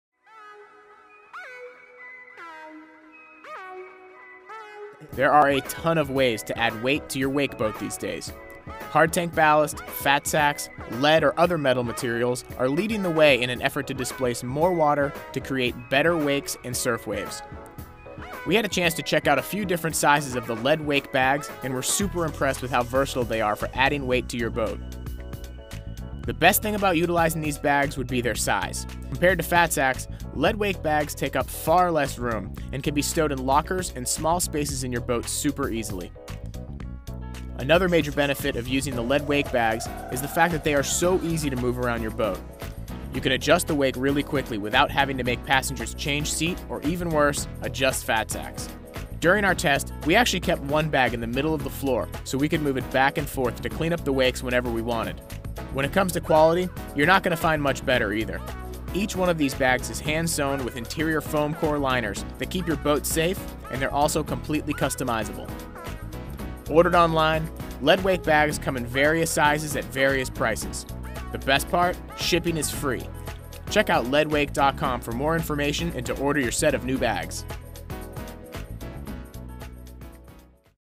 Lead Wake Bags
The versatile and easily adjustable Lead Wake bags are a great way to add weight to your boat. Not only do they take up much less room than ballast bags, but they are completely customizable, making for better surf waves and wakeboard wakes.